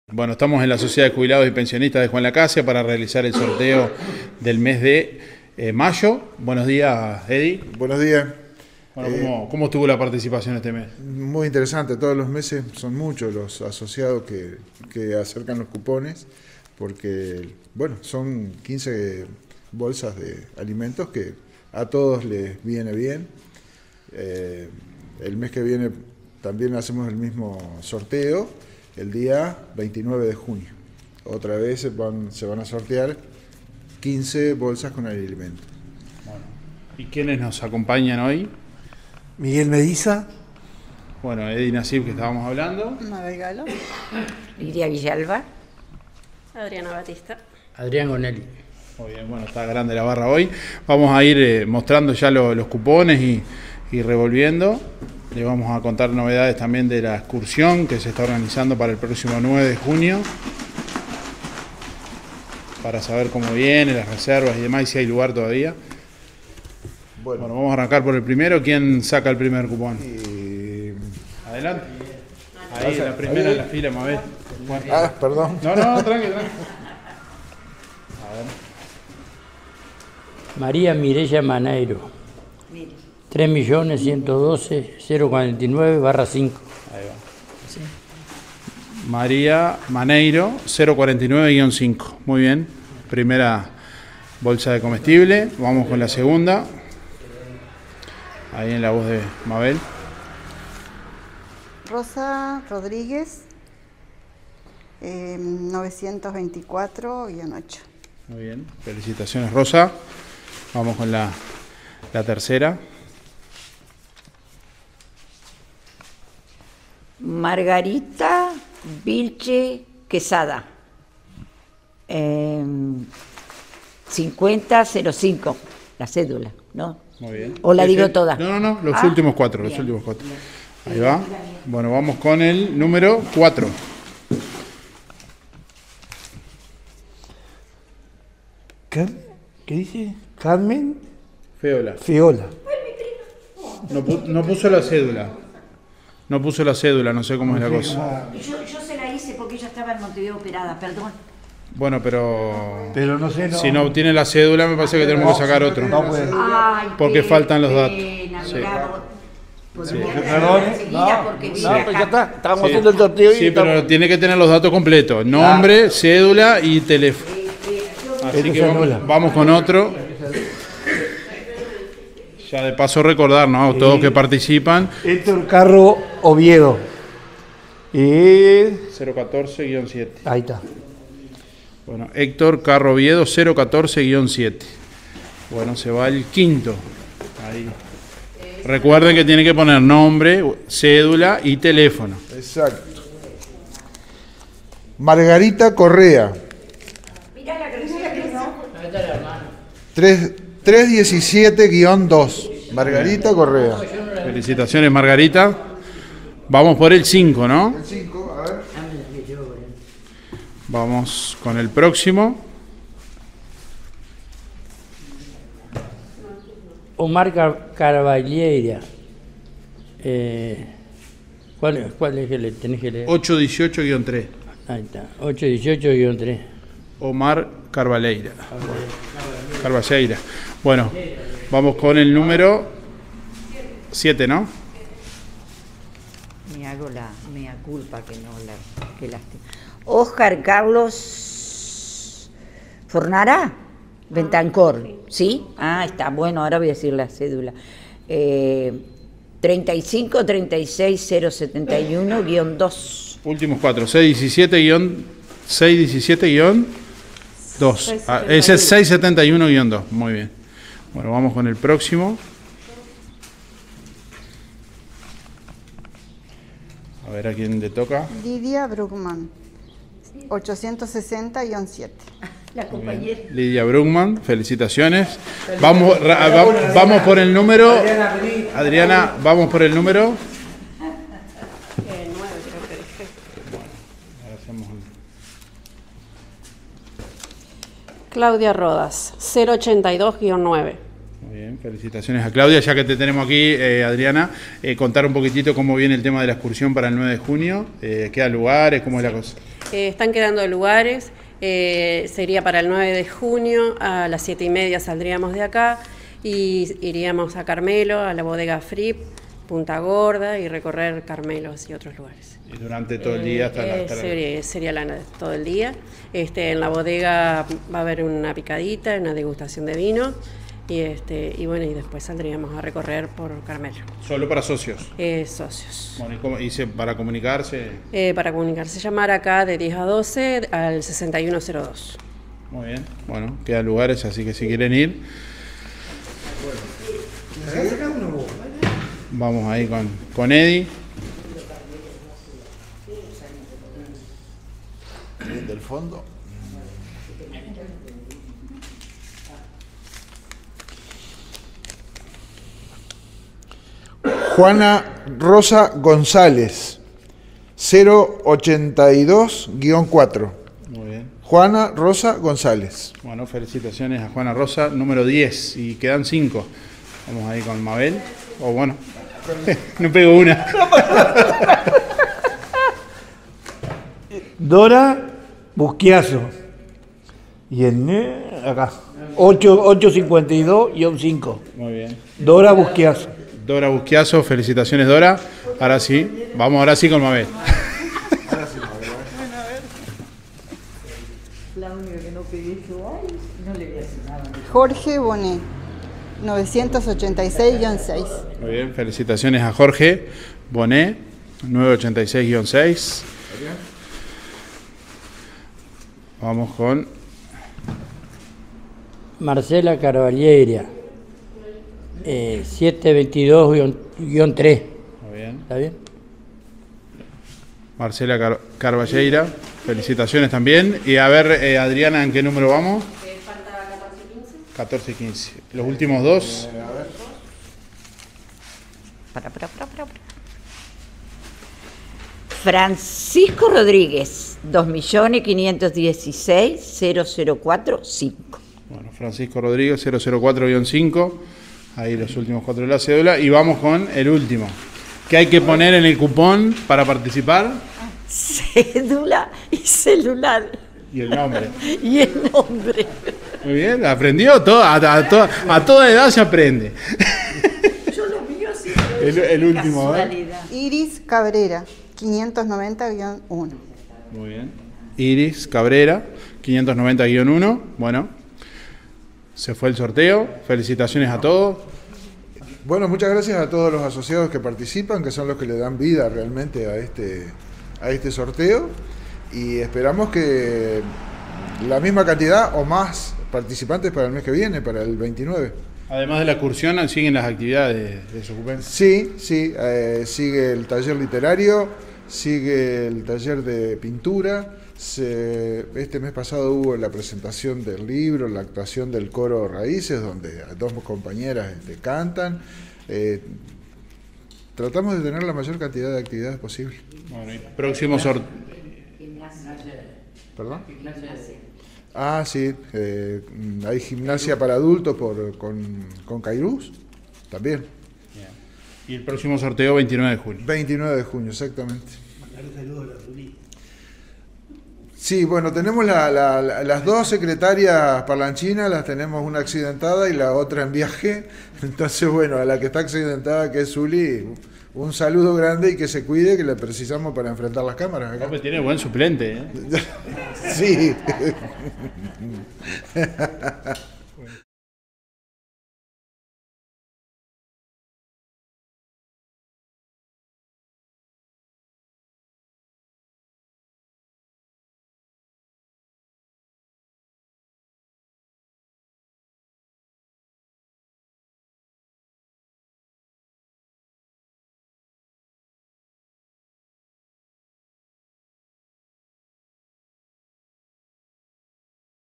Ganadores del Sorteo de SOJUPEN | 30 de mayo
La sociedad de Jubilados y Pensionistas de Juan Lacaze SOJUPEN, realizó el sorteo de 15 bolsas de comestibles entre sus socios.

Participaron todos quienes en el transcurso del mes de mayo depositaron el cupón con sus datos en la sede de Lavalleja 214 y quienes completaron el cupón

Los favorecidos con una bolsa de comestible cada uno/a fueron:

María Maneiro 049-5
Rosa Rodríguez 924-8
Margarita Biche 500-5
Héctor Carro 014-7
Margarita Correa 317-2
Omar Carballeira 818-3
Oscar Fornara 671-2
Lidia Brugman 860-7
Claudia Rodas 082-9
Juana González 082-4
Dora Buschiazzo 852-5
Jorge Boné 986-6
Marcela Carballeira 722-3
Francisco Rodríguez 004-5
Iris Cabrera 590-1

El próximo sorteo se realizará el 29 de junio.